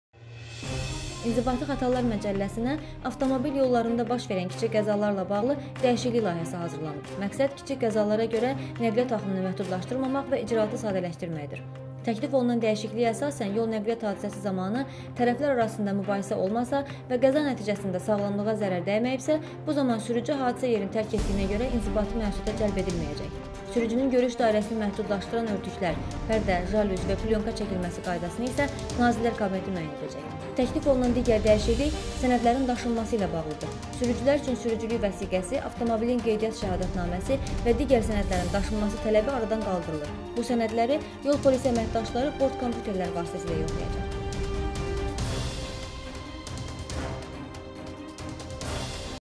Sürücülər sürücülük vəsiqəsini gəzdirməyə məcbur olmayacaq
İnzibati Xətalar Məcəlləsinə avtomobil yollarında baş verək kiçik qəzalarla bağlı dəyişiklik layihəsi hazırlanıb. Məqsəd kiçik qəzalara görə nəqliyyat axınını məhdudlaşdırmamaq və icraatı sadələşdirməkdir.

Təklif olunan dəyişikliyə əsasən, yol-nəqliyyat hasisəsi zamanı tərəflər arasında mübahisə olmasa və qəza nəticəsində sağlamlığa zərər dəyməyibsə, bu zaman sürücü hadisə yerini tərk etdiyinə görə inzibati məsuliyyətə cəlb edilməyəcək.

Sürücünün görüş dairəsini məhdudlaşdıran örtüklər, pərdə, jalüz və plyonka çəkilməsi qaydasını Nazirlər Kabineti müəyyən edəcək.

Təklif olunan digər dəyişiklik sənədlərin daşınması ilə bağlıdır. Sürücülər  üçün sürücülük vəsiqəsi, avtomobilin qeydiyyat şəhadətnaməsi və digər sənədlərin daşınması tələbi aradan qaldırılır.  Bu sənədləri Yol Polisi əməkdaşları bort-kompüterlər vasitəsilə yoxlayacaq.